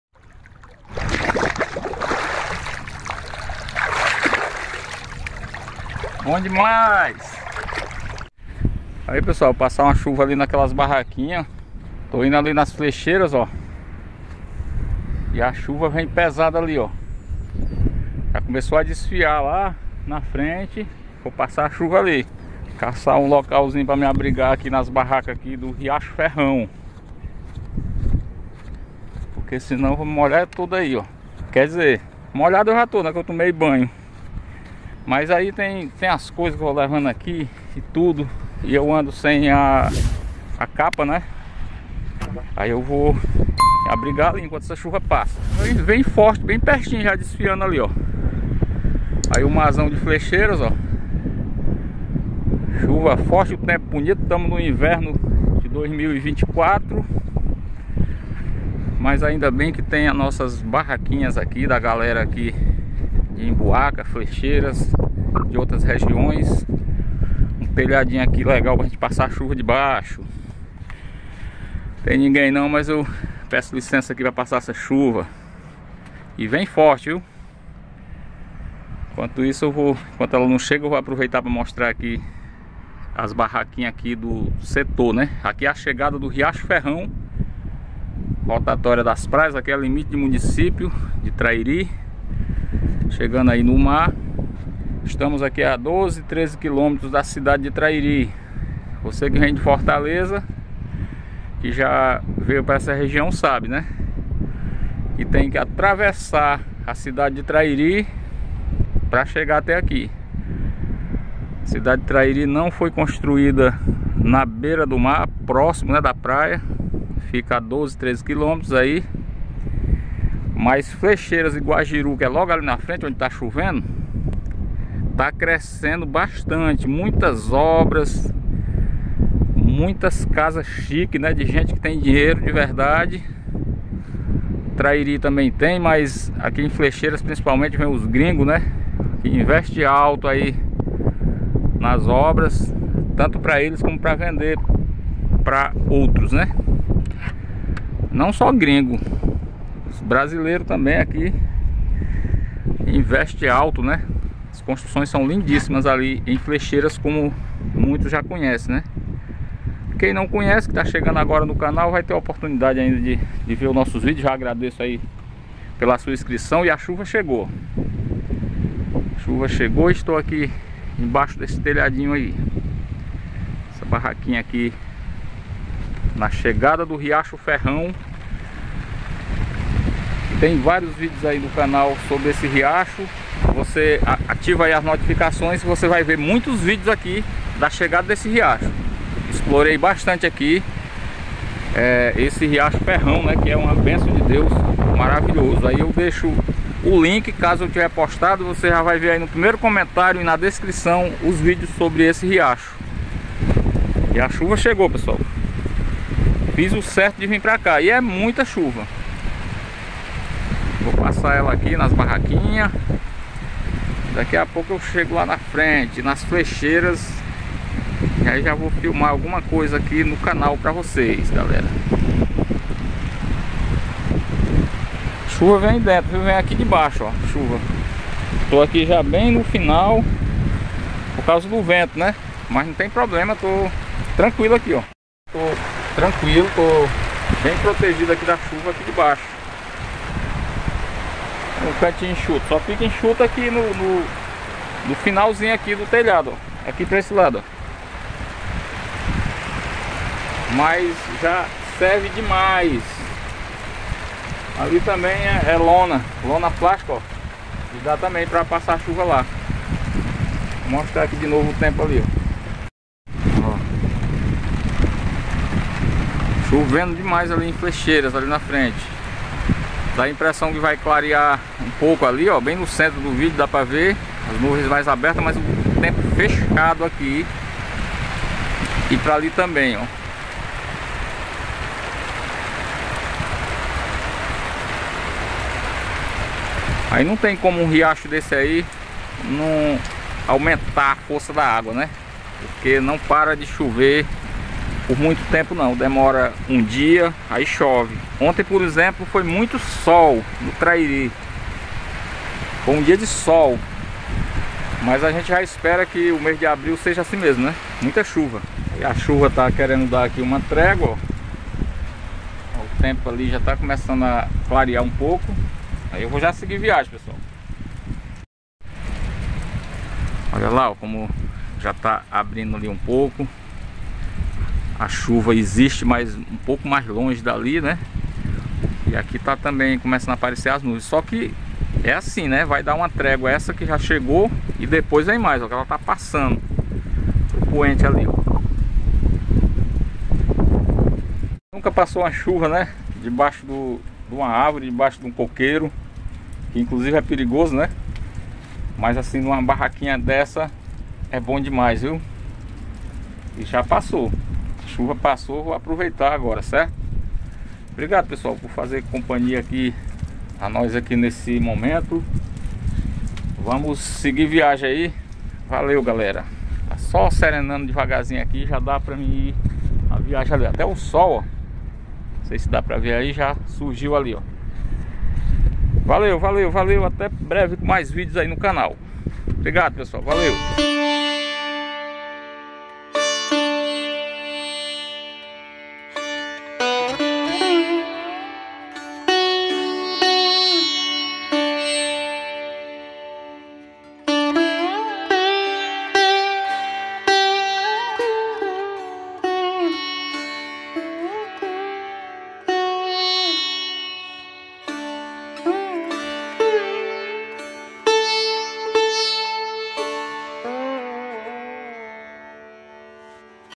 ME ABRIGUEI DA CHUVA FORTE NAS BARRACAS DO RIACHO FERRÃO INDO PRA FLECHEIRAS
A caminho de flecheiras,  tive que me abrigar da chuva . Fui até as barracas do riacho Ferrão e deu tudo certo